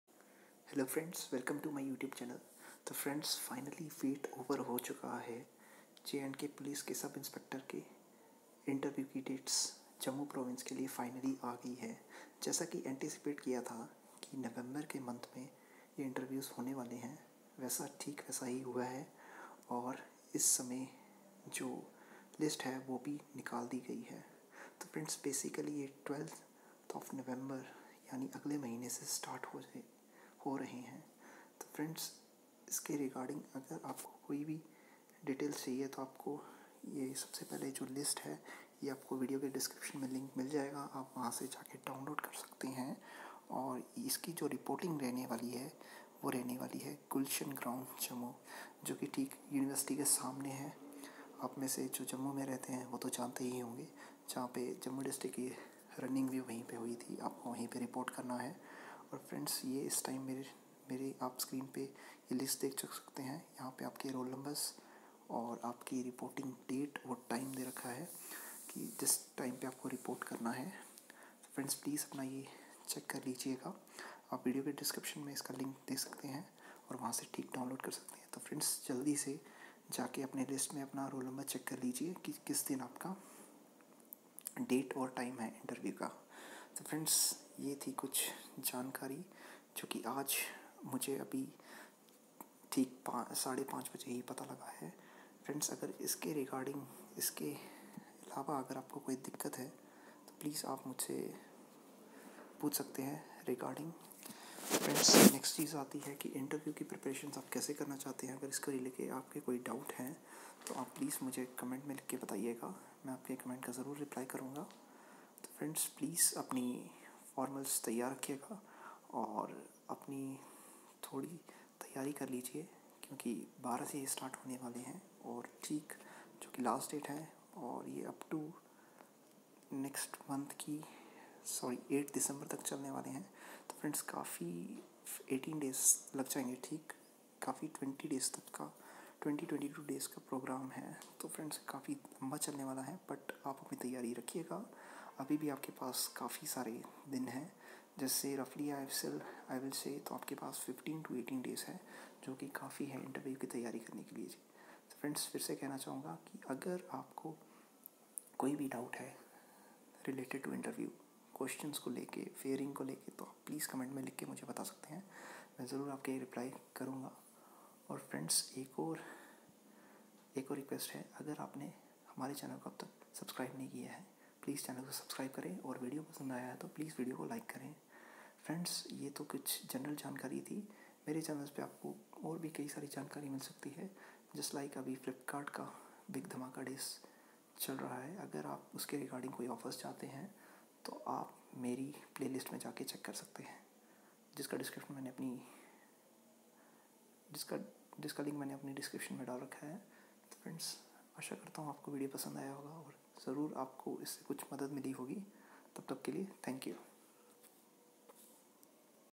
JK POLICE SI INTERVIEW VIVA VOICE DATES JAMMU PROVINCE
Candidates finally  wait has  been over
Check out the dates of jkpolice SI INTERVIEW from link given below

If you have still any doubt related to preparation and dress code please ask me in comment section.

Friends you can also get some online offers from my videos please check my playlist from the link given below